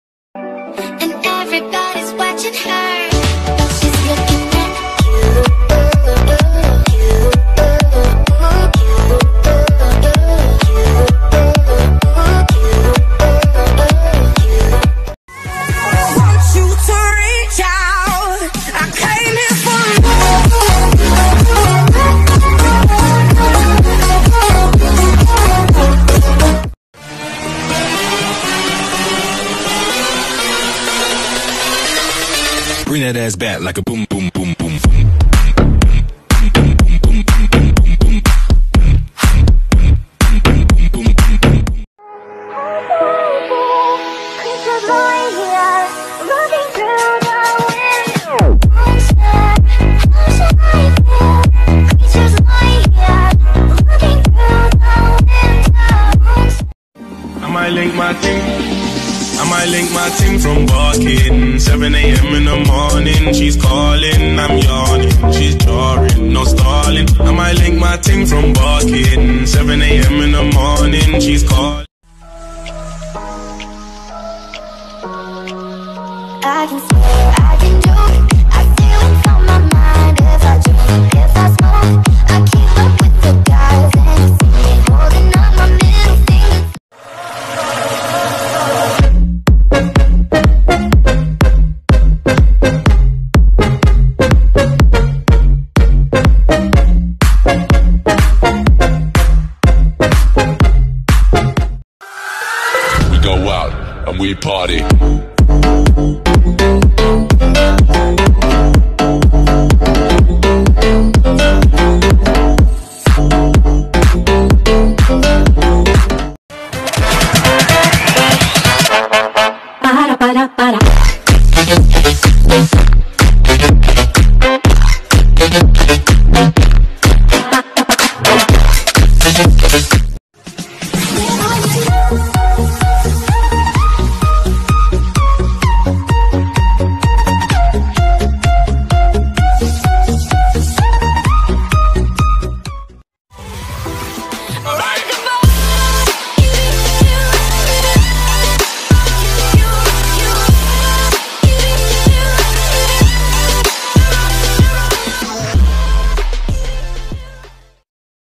FINGER SHUFFLE DANCE Challenge Compilation Musically 2018 | ANTHONYNVOKER
Finger Shuffle Dance Challenge Es el mejor Musicalmente " Pueden Compartir el Video :'D SI ESTE VIDEO LLEGA A LOS #30 LIKES SUBO LA SEGUNDA PARTE ! VAMOS VAMOS GENTE! A REVENTAR CON LOS LIKES" 🖒❤🖒